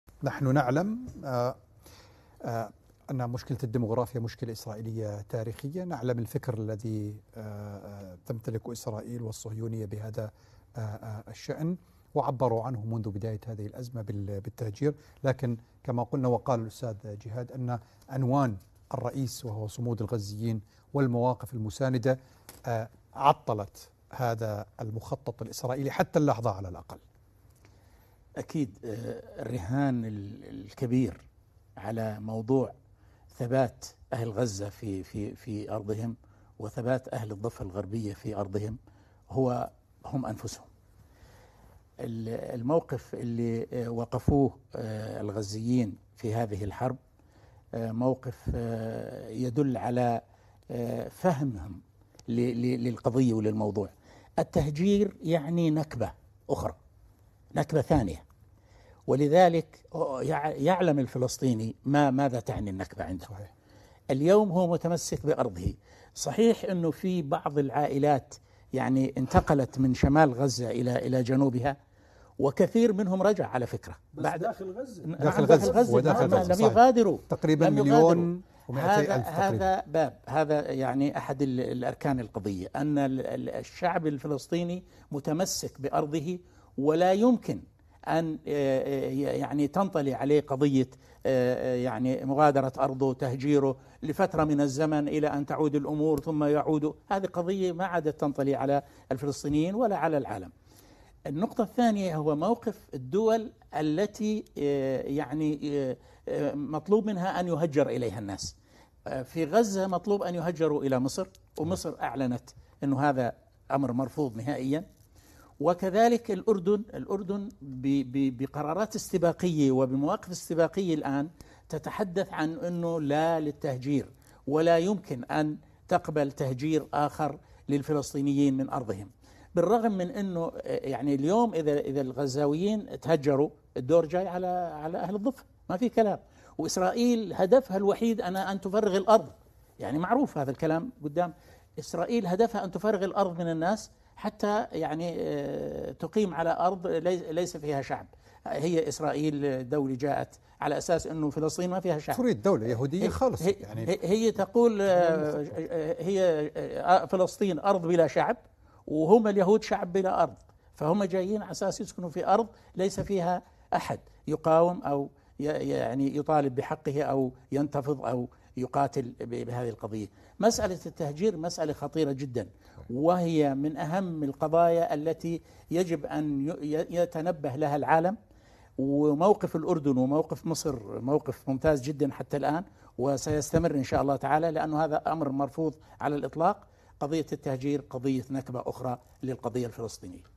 تغطية خاصة | غزة في المواجهة رئيس الهيئة الادارية في جمعية المركز الاسلامي الدكتور جميل دهيسات
- الشعب الفلسطيني متمسك بالأرض ولا يمكن ان يفكر في تركها
- الأردن موقفه ثابت من القضية الفلسطينة ويرفض بشكل قاطع تهجير الفلسطينيين
- الاحتلال هدفه الوحيد ان يفرغ فلسطين من الشعب